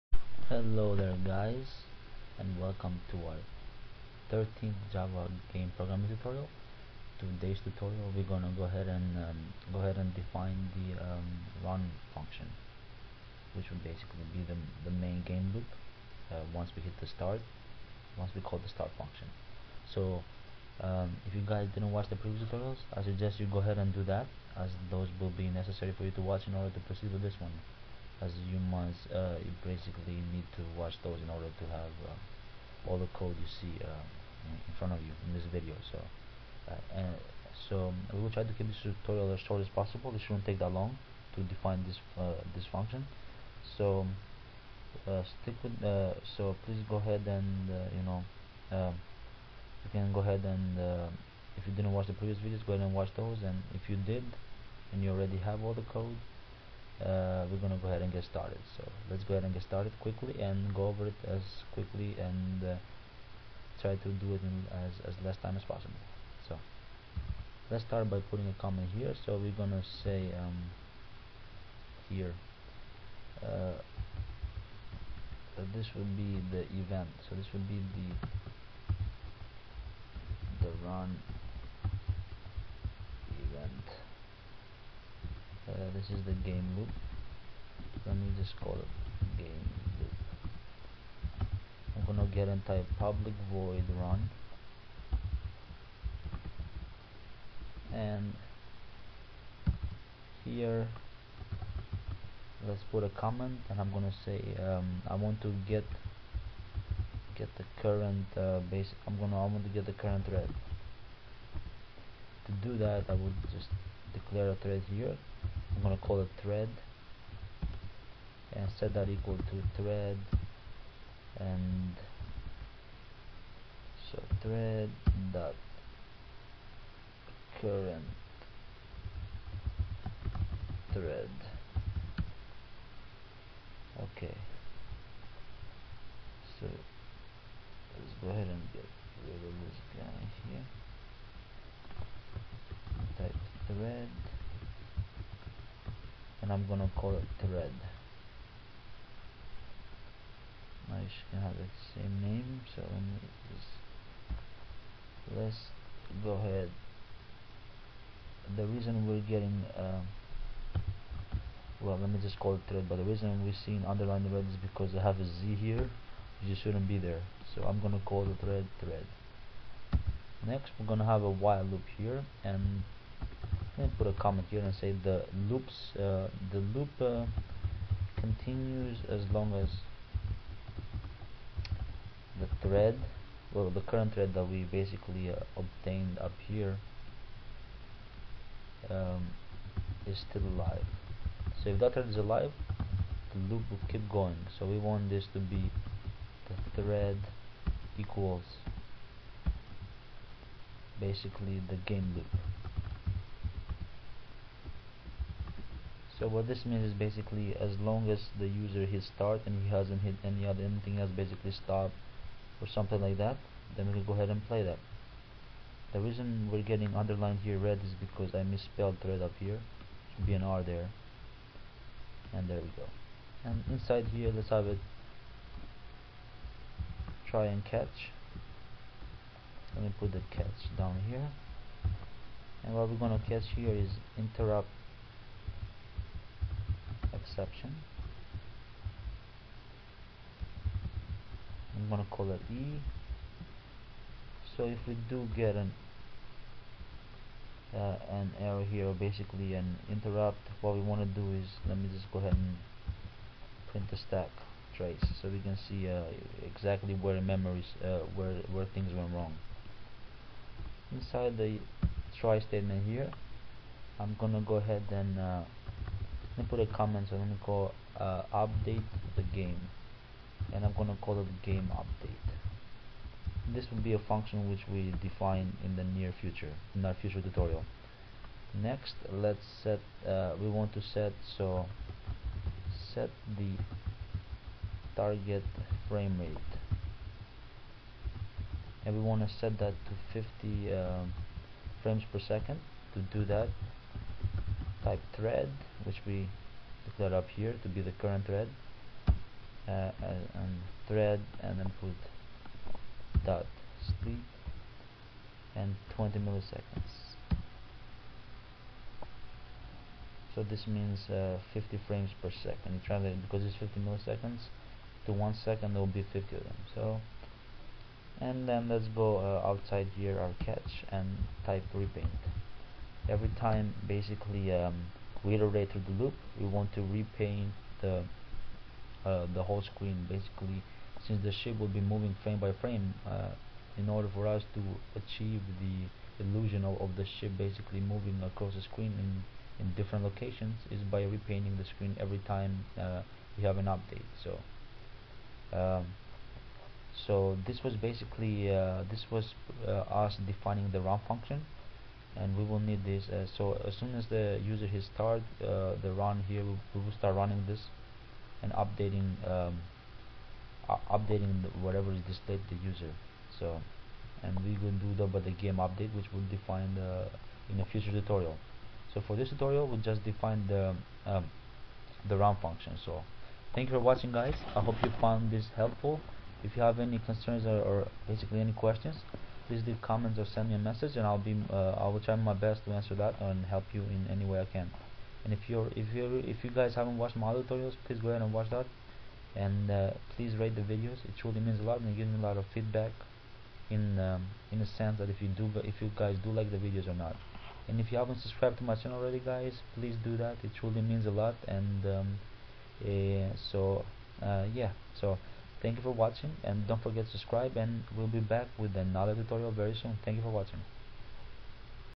Java Game Programming Tutorial For Beginners Asteroids Pt .13 - Asteroids run())
This is the 13th java game programming tutorial which will be followed by many more. This part we will be defining the main Asteroids Class for the Asteroids game.